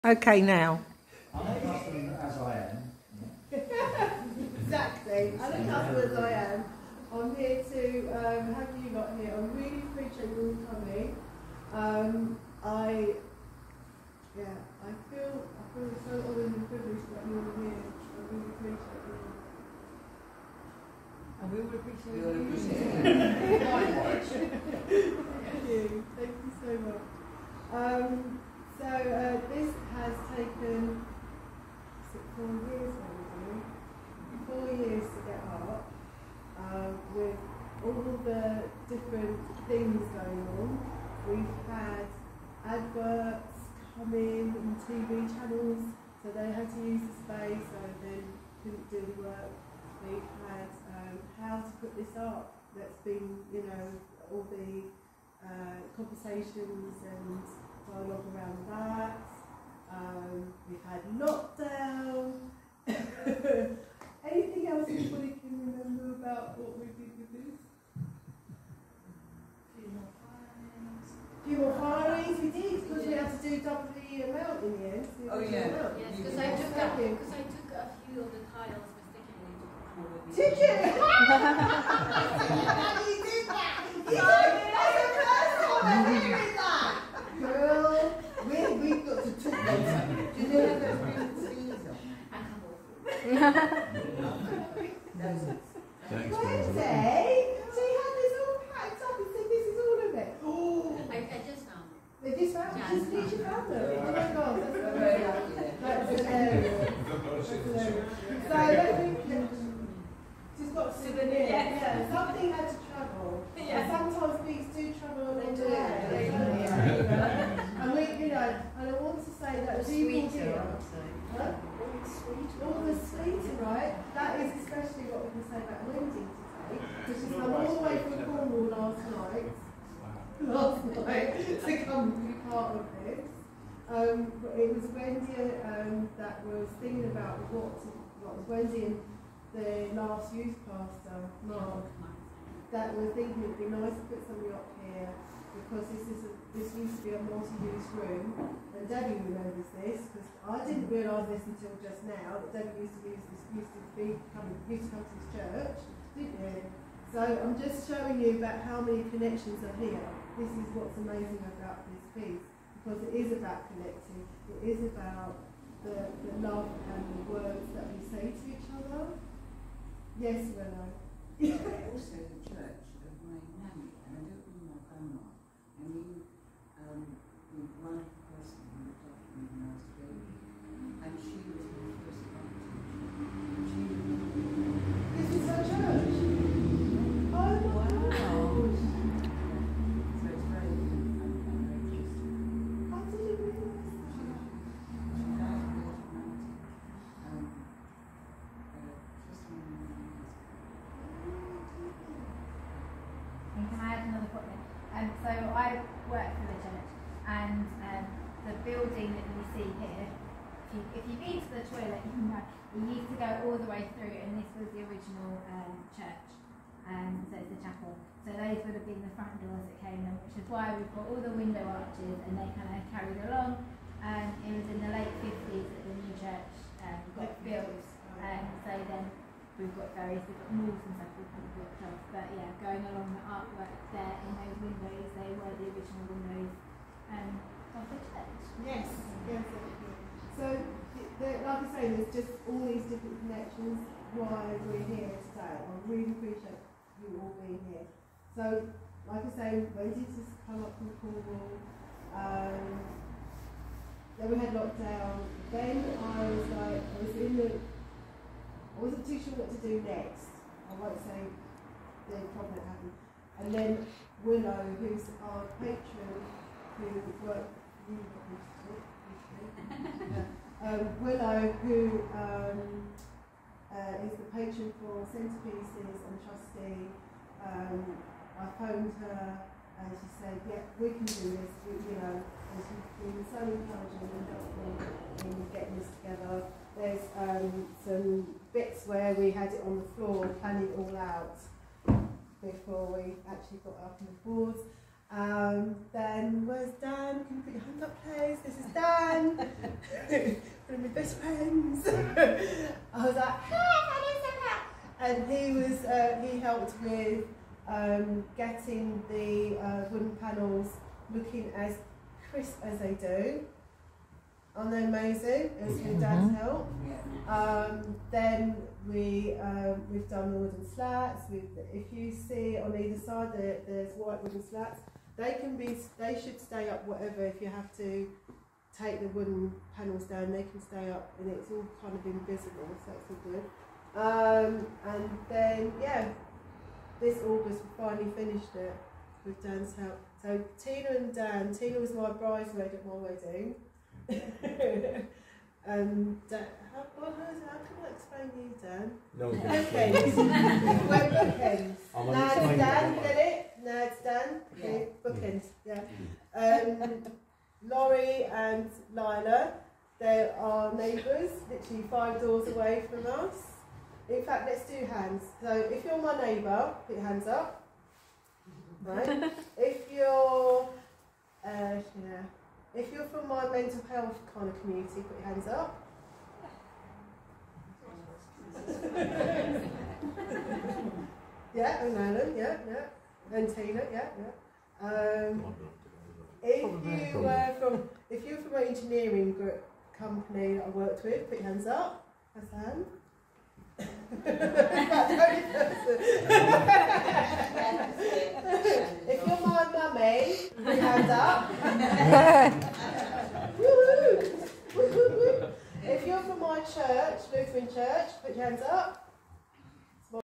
Bee Installation at St. Paul’s, Crofton, Orpington. BR6.
Recorded Sunday 22 October 2023.
Uploaded Monday 23 October 2023.

Interpersonal Intelligence manifested at its best!

Not the whole speech and a good bit of it recorded. It wasn’t just me who did the installation. It was also all the people to whom I am connected. Not everyone could be there and I feel blessed and thankful for the many more relationships I have in my life. The friends and family who appear on this video are just a representation of the many amazing lovely people in my life.

Thank you God for Your blessings you have showered on me and the people around me.